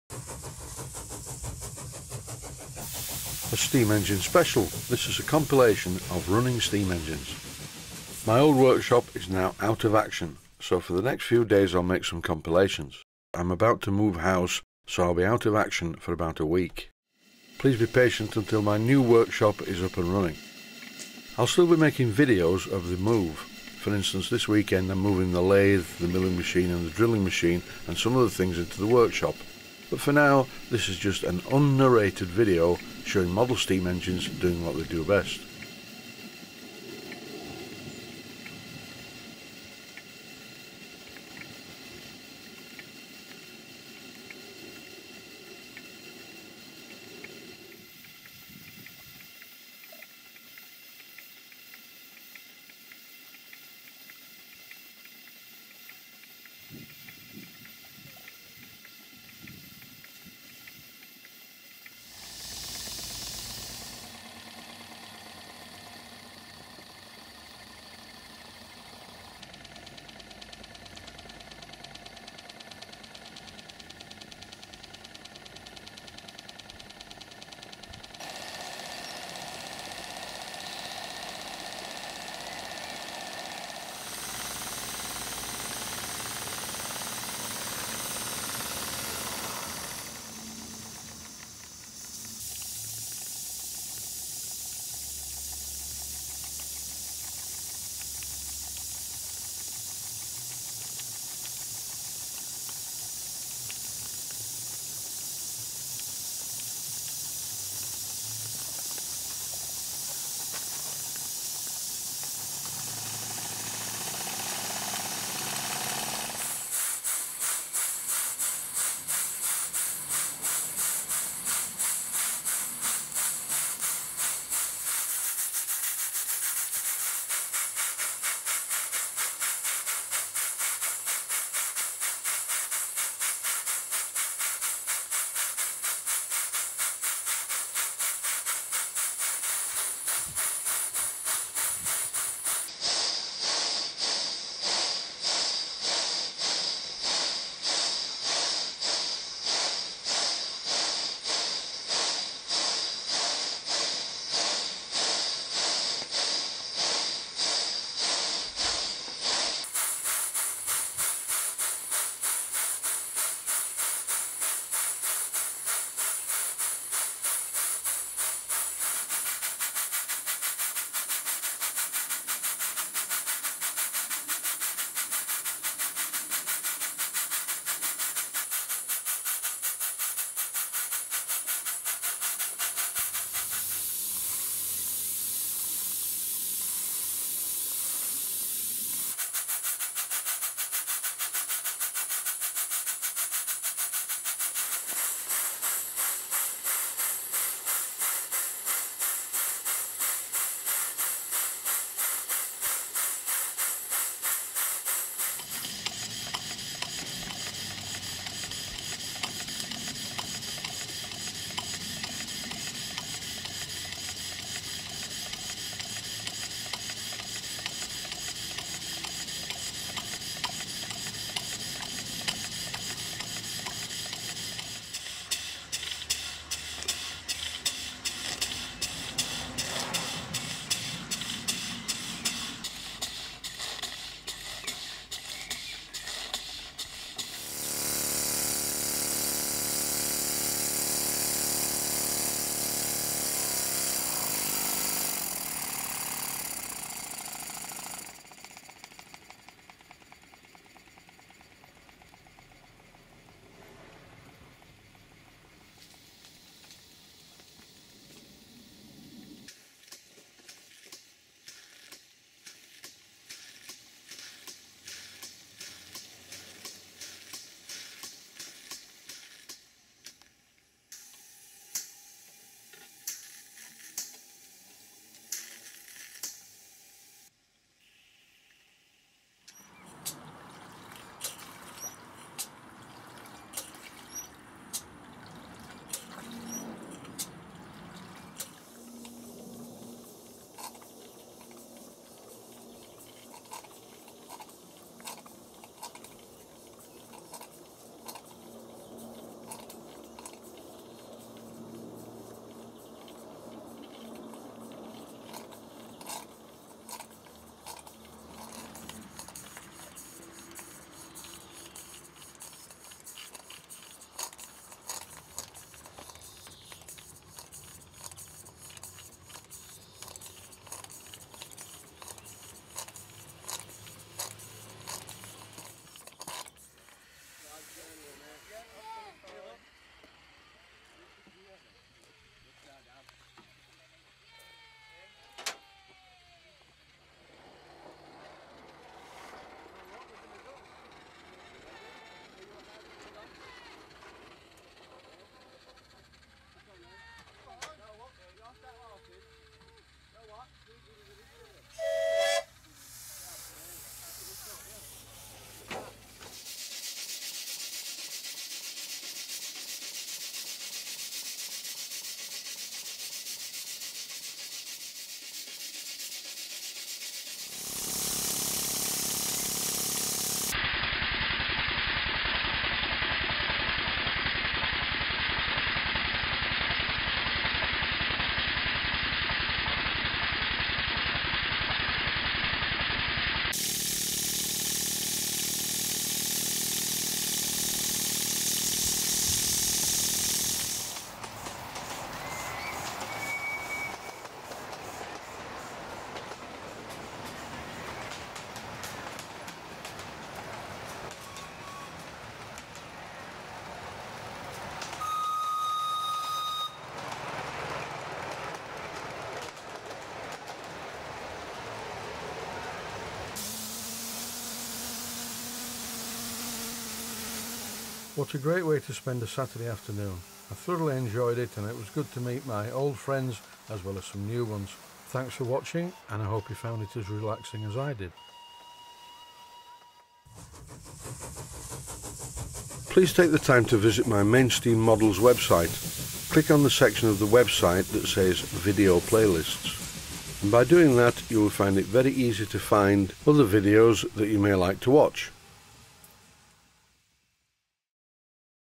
MODEL STEAM ENGINES IN STEAM
Model Steam Engines In Steam. This is a compilation video showing model steam engines doing what they do best.